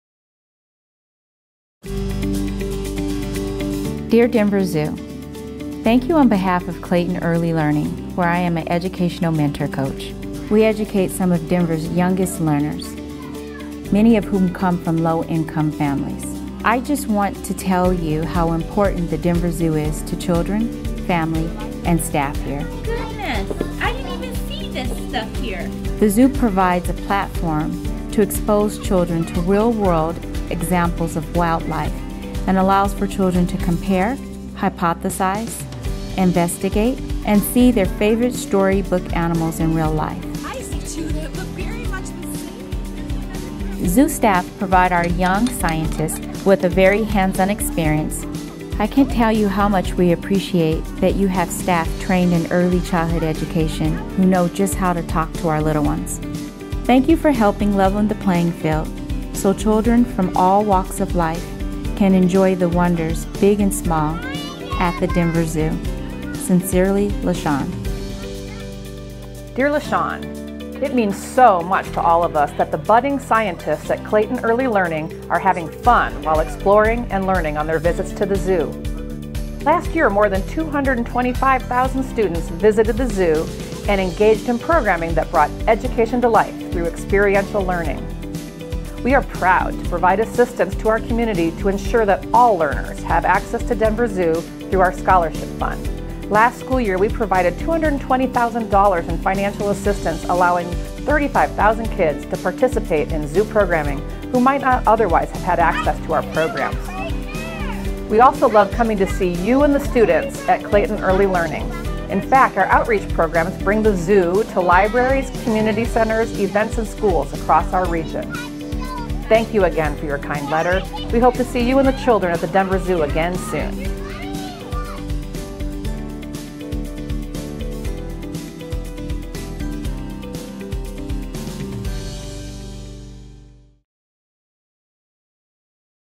Denver Zoo provides access of informal science education to people of all walks of life.
Denver Zoo is pleased to provide access of informal science education to people of all walks of life.